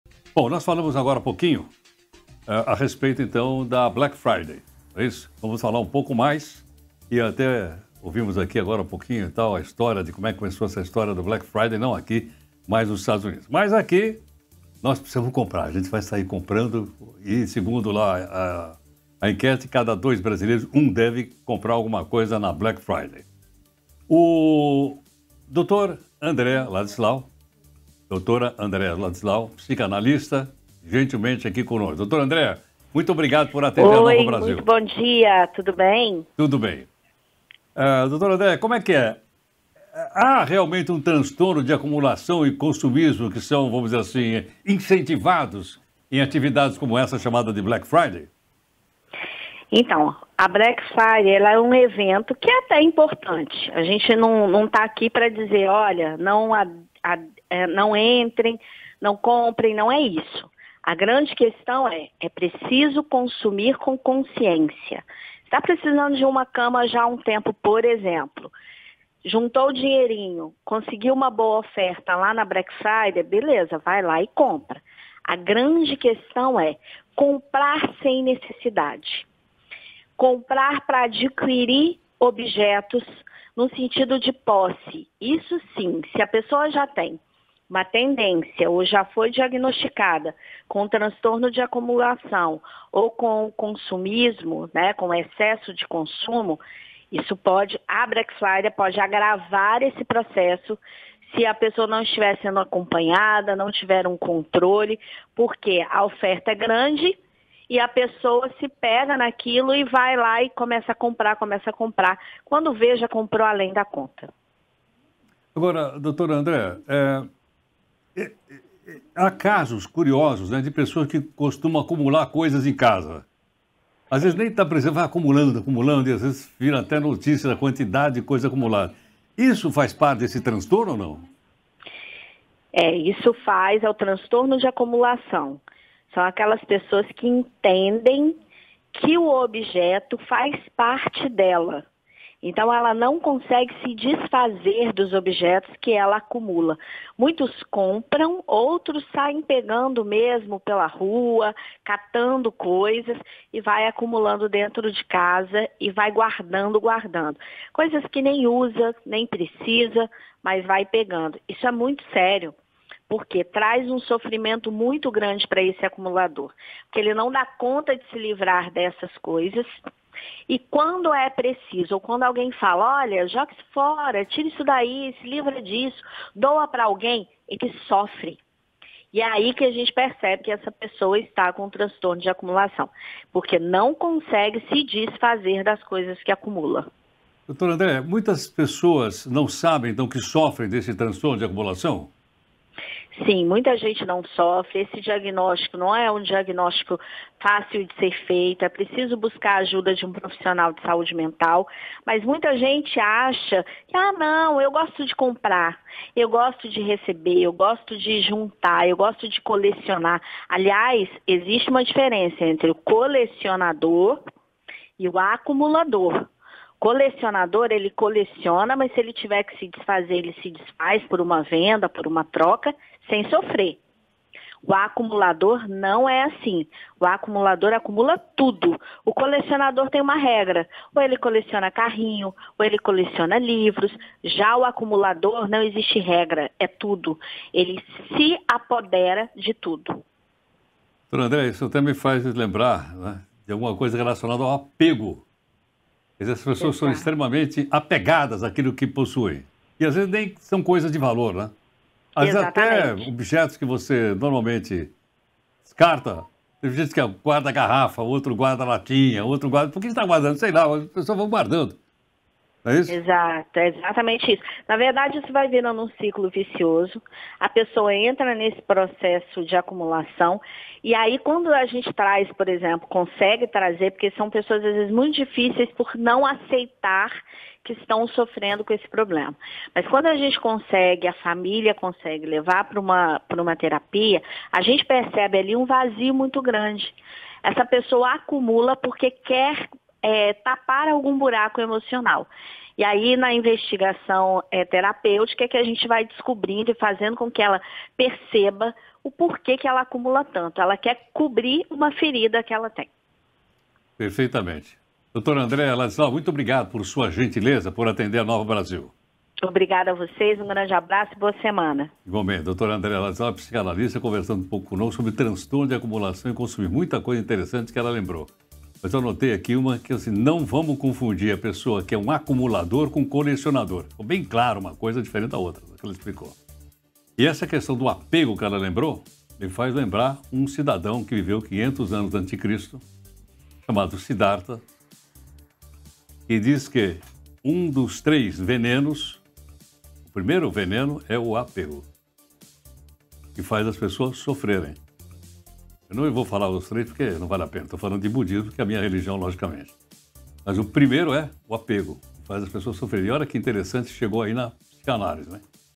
A psicanalista Andréa Ladislau fala sobre o transtorno de acumulação e o consumismo na Black Friday
Em entrevista com Heródoto Babeiro, a psicanalista, Andréa Ladislau, fala sobre o transtorno de acumulação e o consumismo na Black Friday.